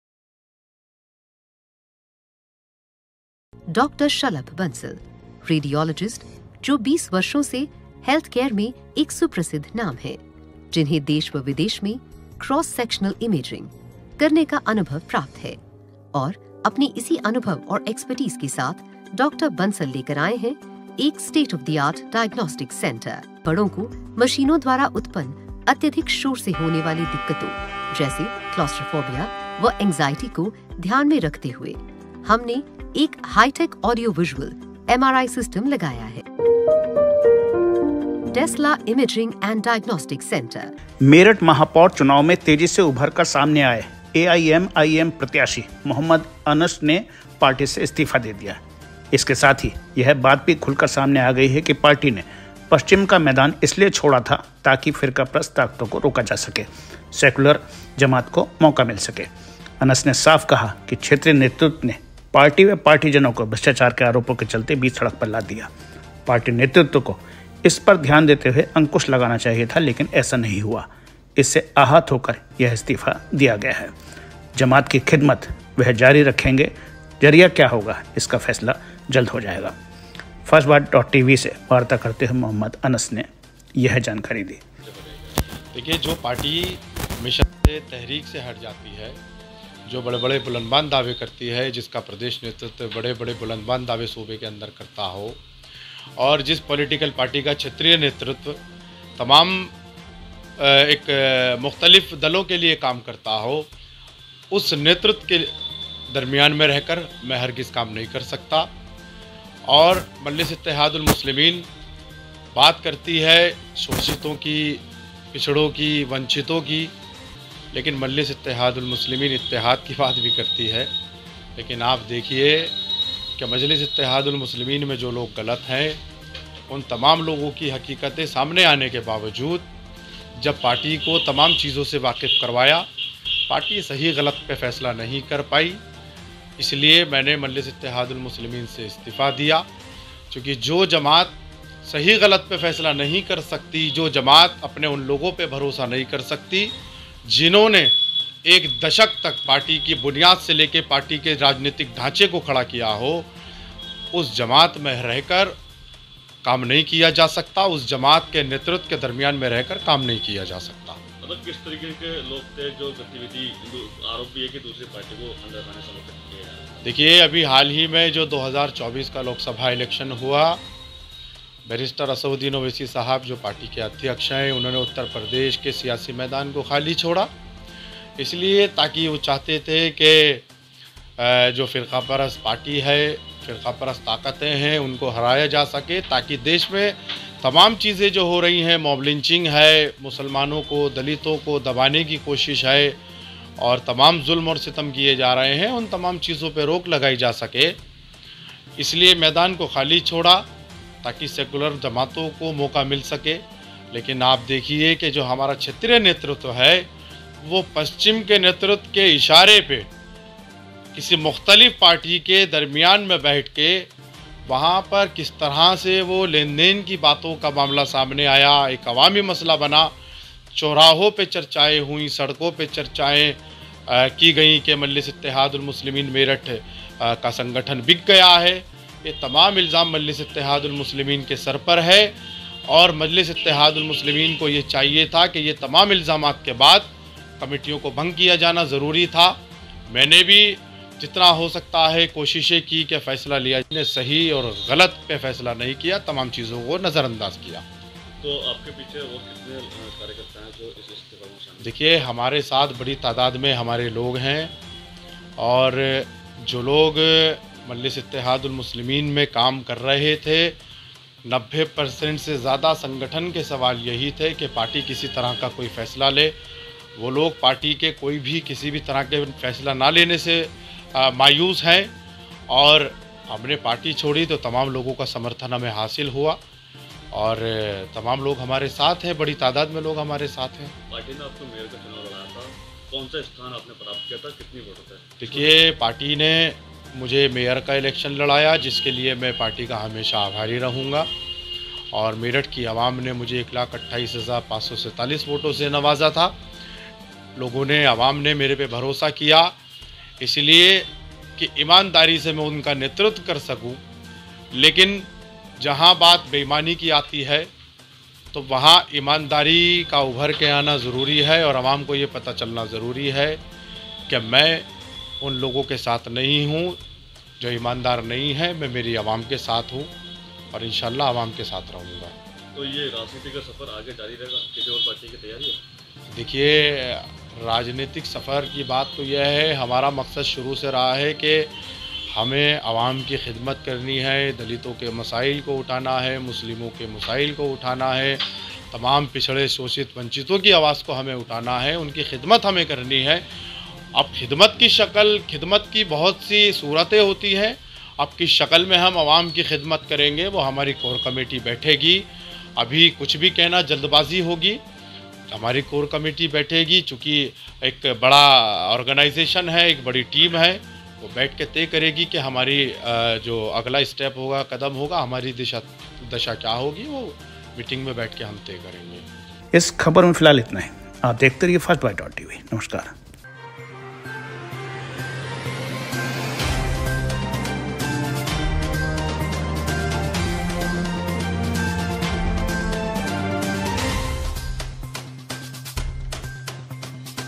मोहम्मद अनस ने एआईएमआईएम को क्यों कहा अलविदा First Byte.tv
मोहम्मद अनस ने एआईएमआईएम को क्यों कहा अलविदा
एआईएमआईएम को मोहम्मद अनस ने कहा अलविदा,
महापौर चुनाव में दूसरे स्थान पर रहे थे अनस,
पश्चिम में प्रत्याशी न उतारने की भी अनस ने बताई वजह,
फिरकाप्रस्त ताकतों को रोकने के लिये छोड़ा मैदान,
क्षेत्रीय नेतृत्व की गलत नीतियों ने पार्टी को नुकसान पहुंचाया,
पार्टी नेतृत्व को एक्शन लेना था लेकिन नहीं लिया-अनस,

1.वाकई मोदीजी की तुलना नेहरूजी से नहीं की जा सकती